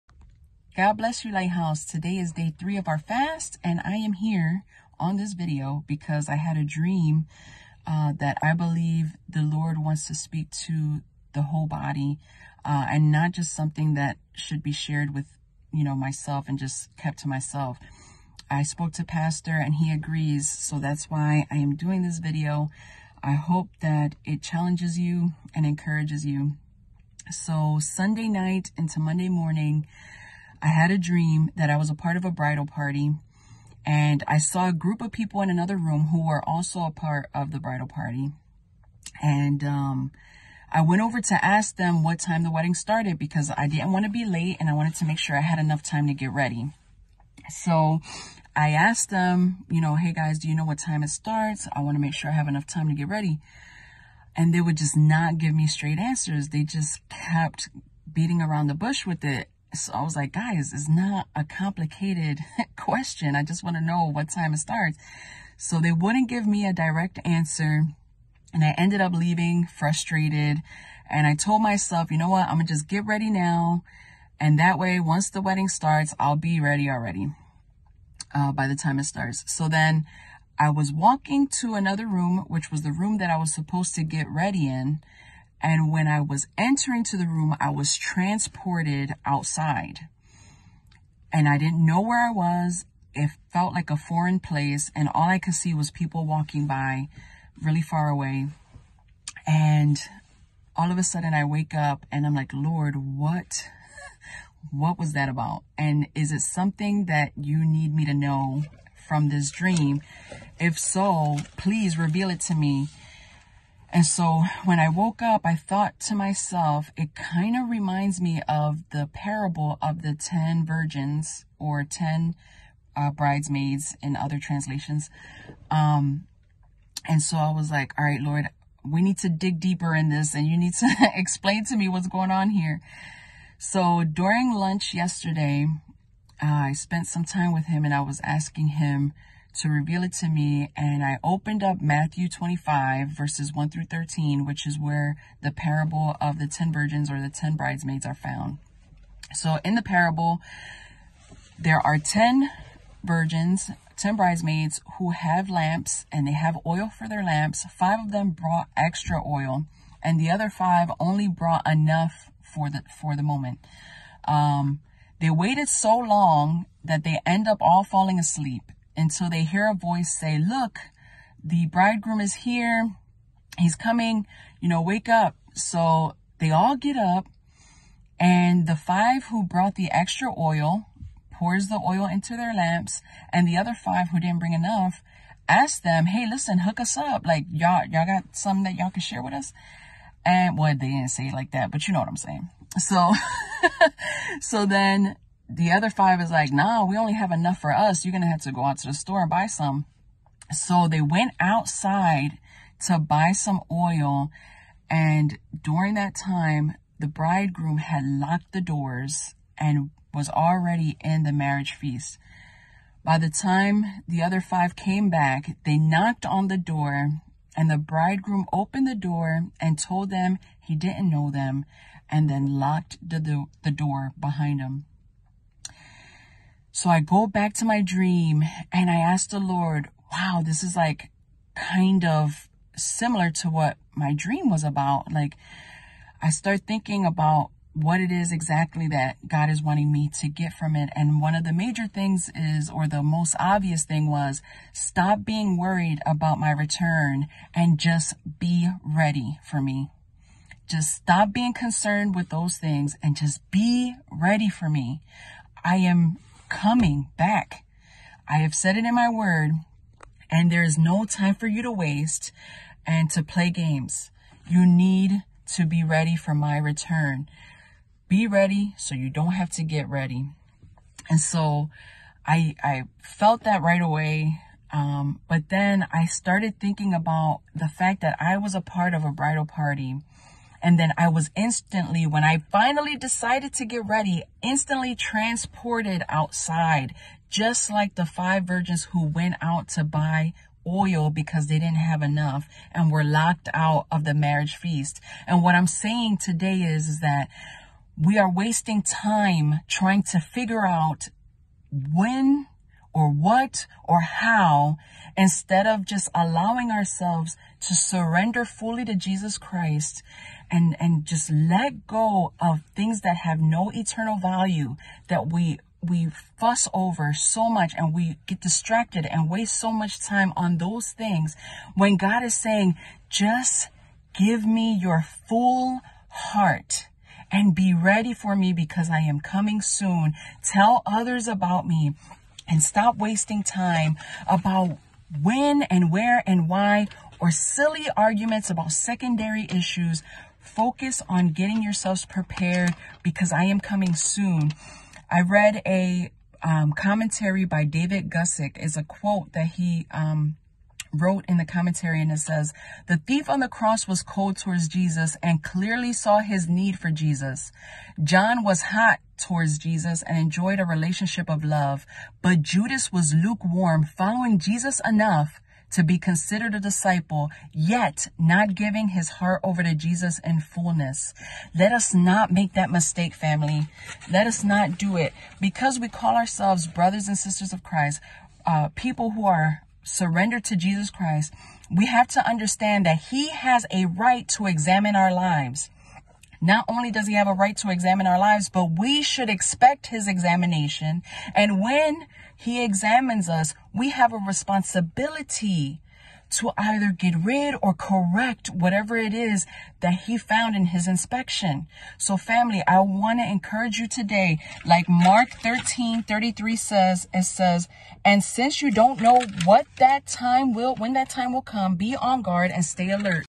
When's the wedding!?!?- Vanessa Morales Fasting Day 3
Sister Vanessa shares a dream she had as the church fast started...